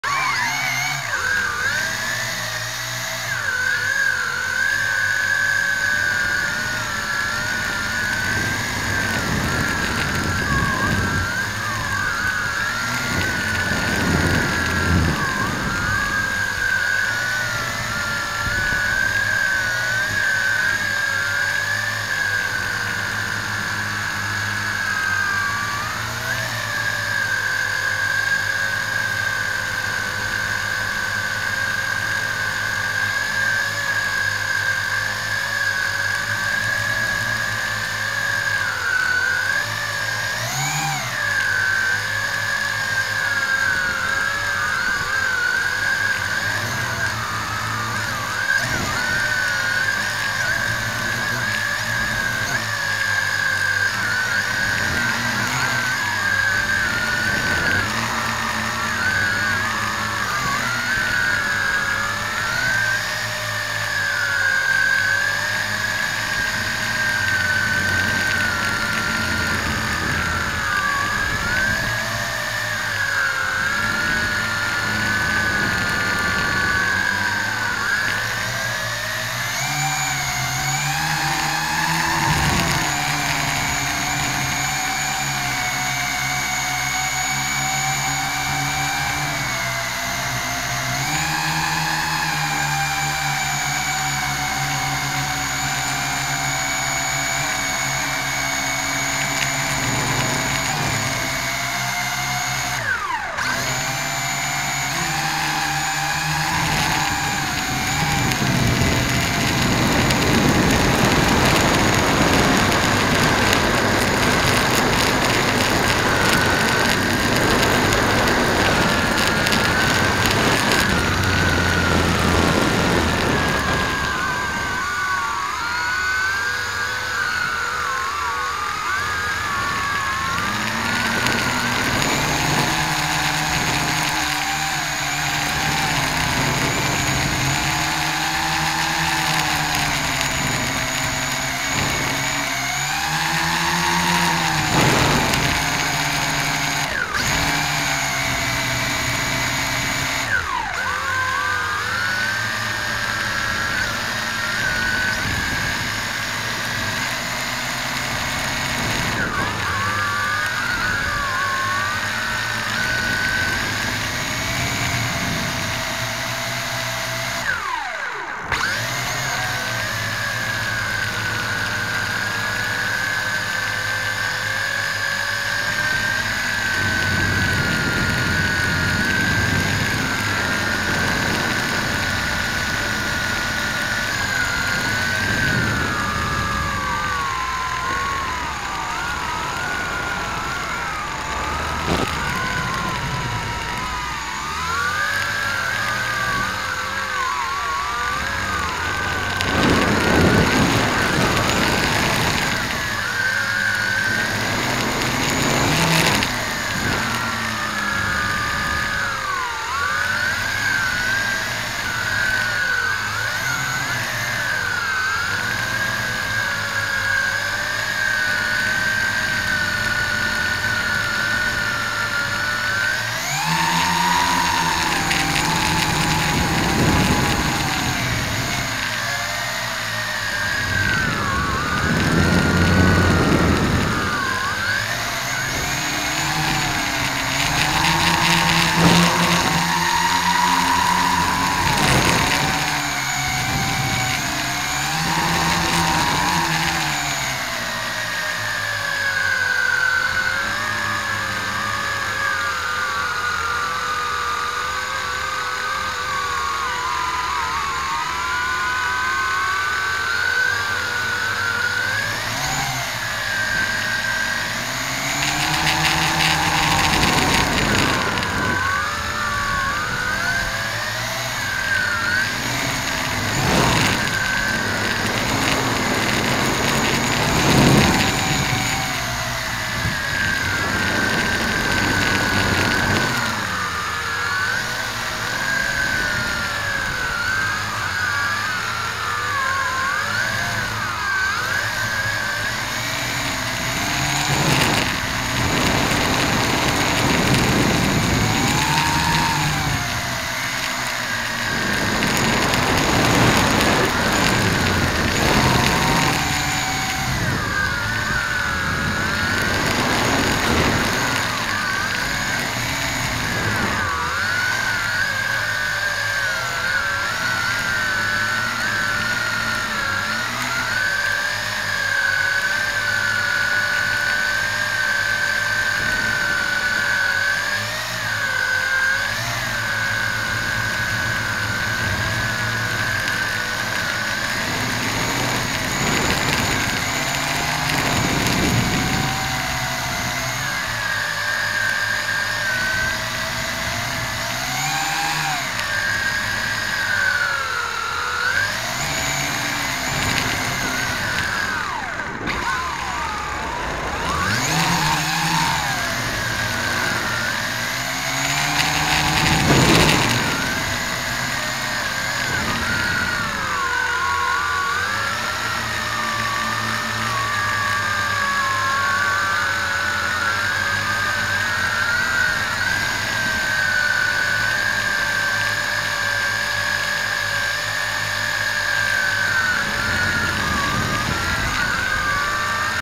Flying Quadcopter - Tarantula X6
Zooming around with my Tarantula X6